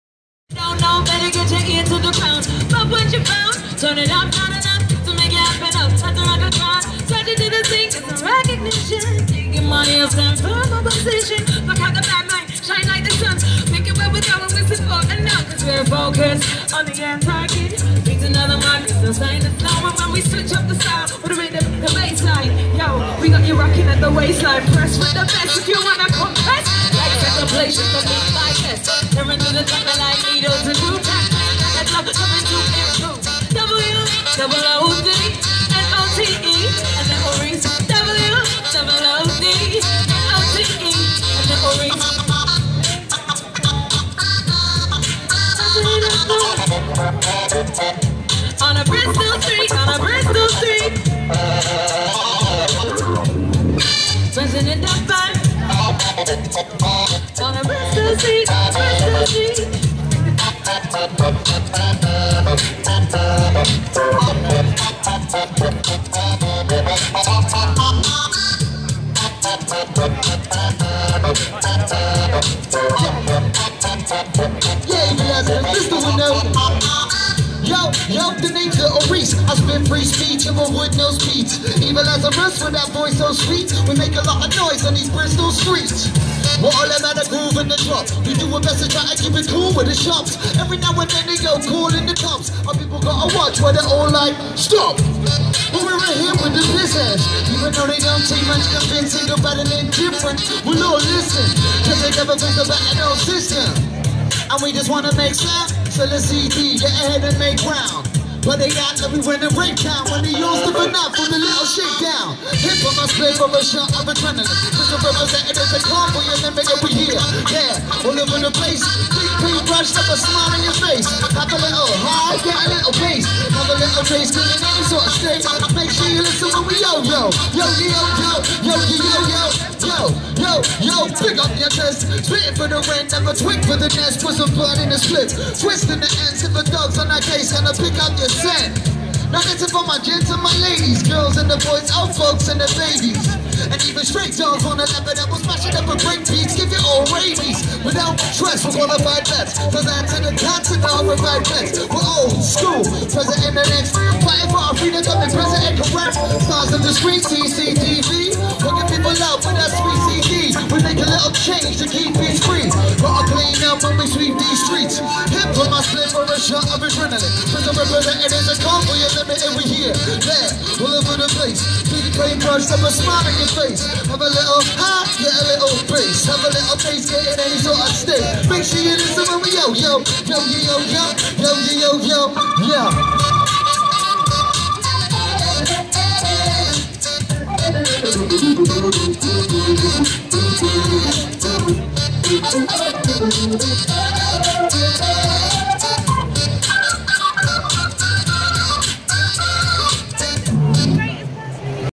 Mr Woodnote feat. Eva Lazarus, Lil Rhys
Street performance in Cabot Circus, Bristol, August 2014